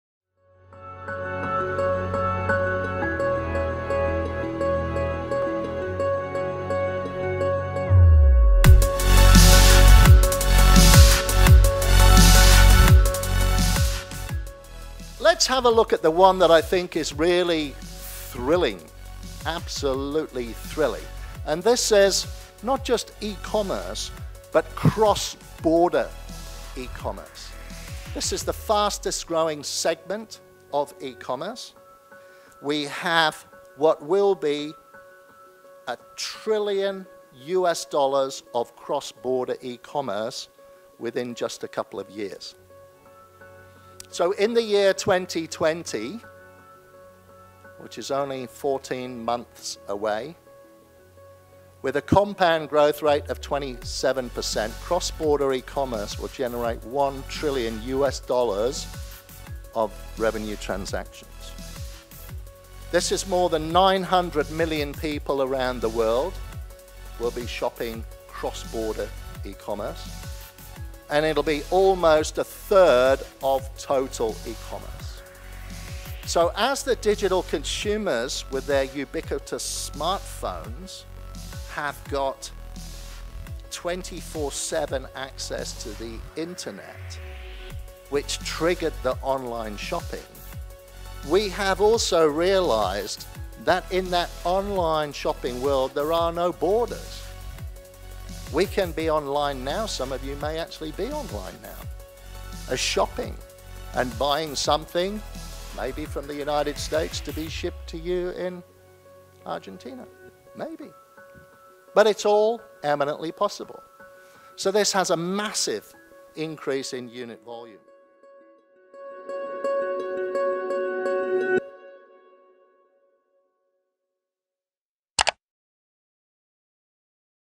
GAA Exclusives: Changing Airfreight Landscape by Mark Millar – Cross Boarder e-Commerce
Mark Millar presenting at the Global Aircargo Alliance Kick-Off Meeting that was held in Bangkok, Thailand last October 22-25, 2018.